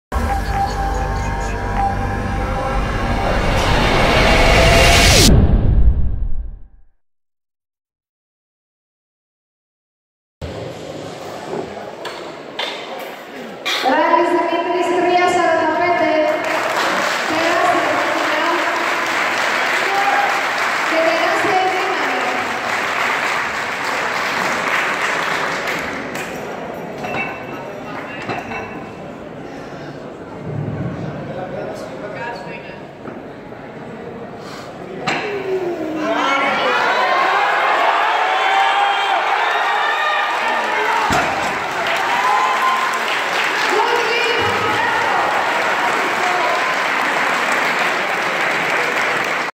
Rallis Dimitris 345 kg/760 lbs Deadlift . GPA/IPO 2017 European Powerlifting Championship
Rallis' Dimitris 1st attempt at 345 kg /760lbs .Good Lift.
GPA/IPO European Powerlifting Championship 2017.
Athens/Greece.